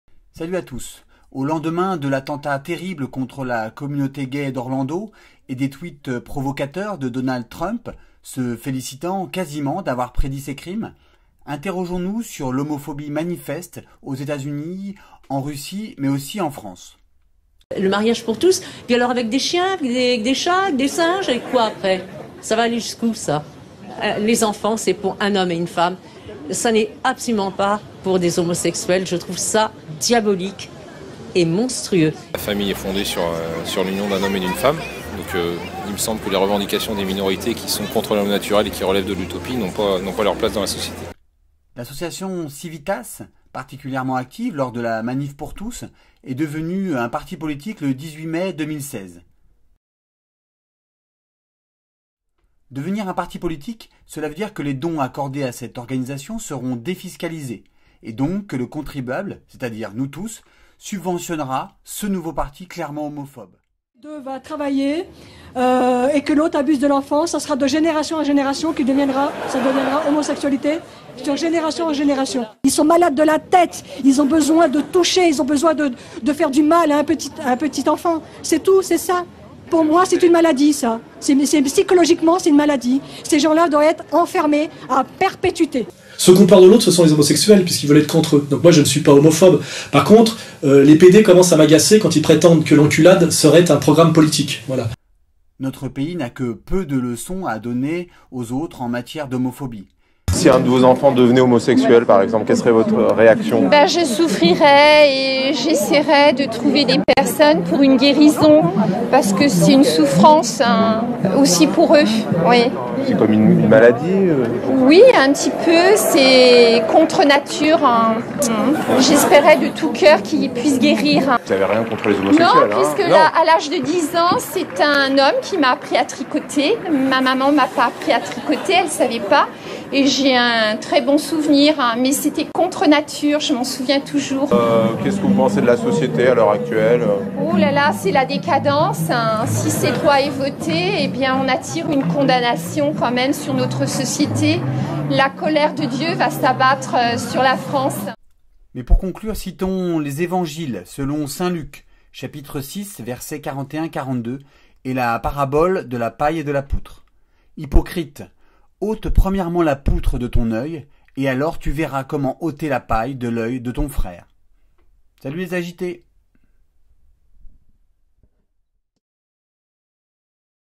Un nouveau parti politique homophobe en France !
Depuis le 12 juin, jour des attentats d'Orlando, le mouvement CIVITAS est devenu un parti politique. Radicalement homophobe, écoutons les paroles de ses militants et combattons l'homophobie partout: Orlando, Moscou, Raqqa, Paris...